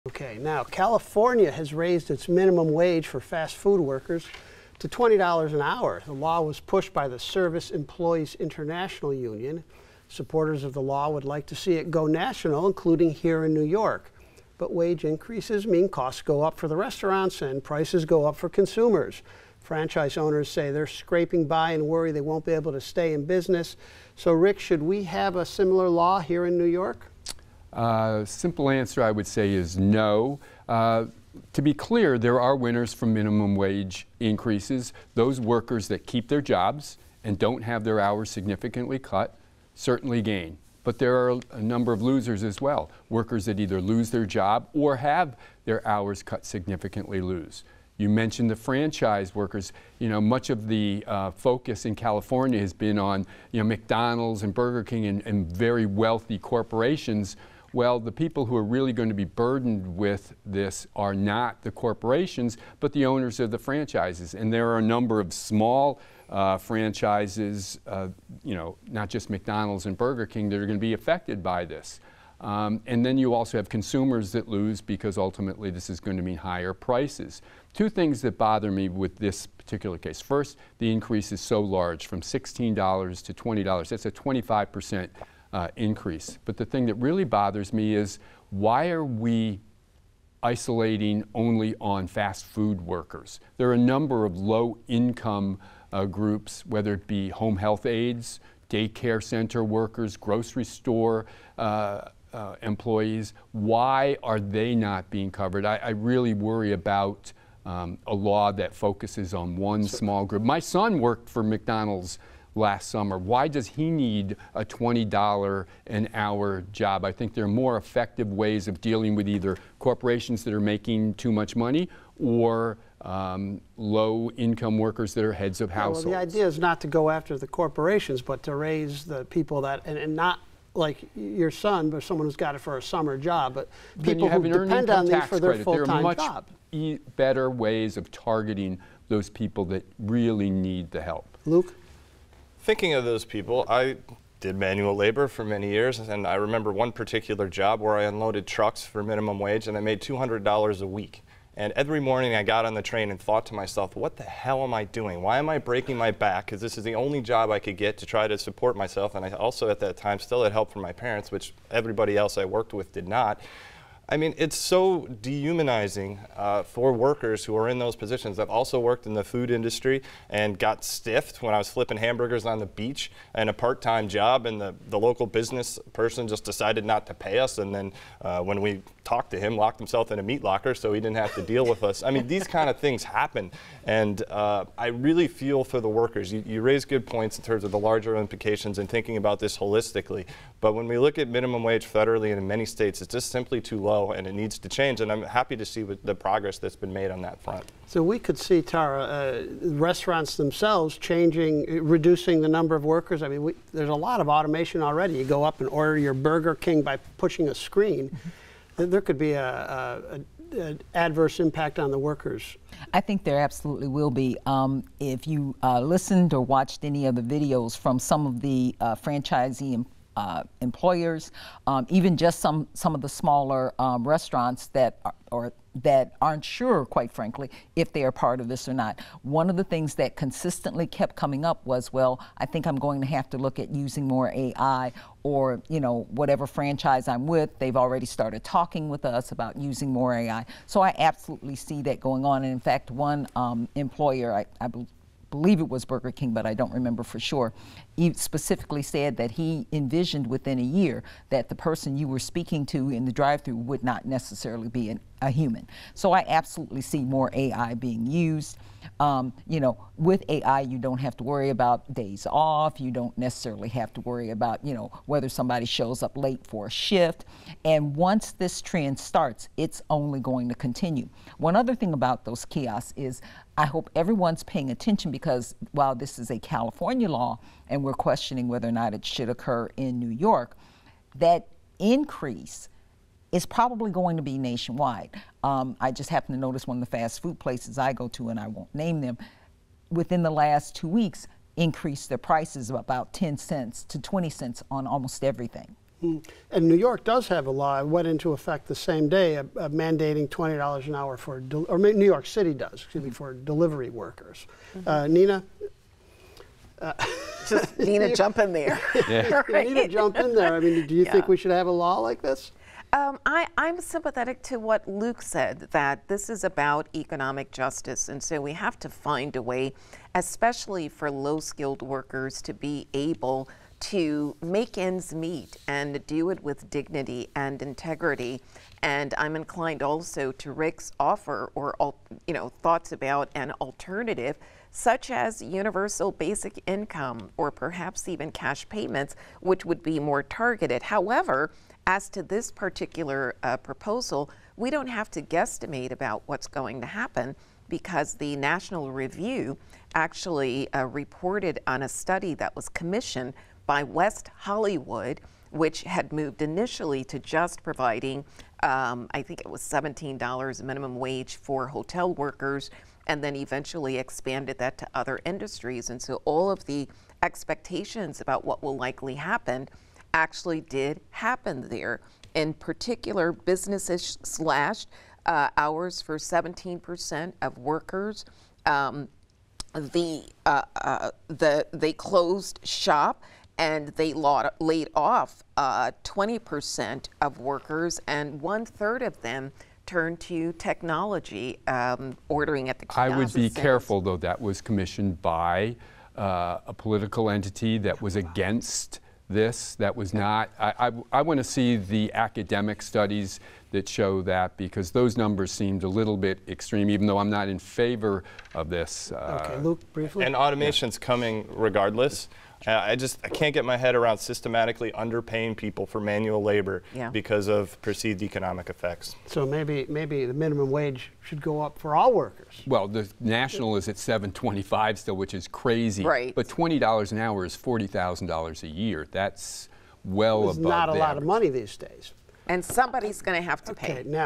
Should New York Increase the Minimum Wage Like California | Ivory Tower
David Chanatry, Rick Fenner, and Luke Perry of Utica University, Nina Moore of Colgate University, and Tara Ross of Onondaga Community College discuss whether or not it would be wise for New York to follow California's lead in increasing the minimum wage.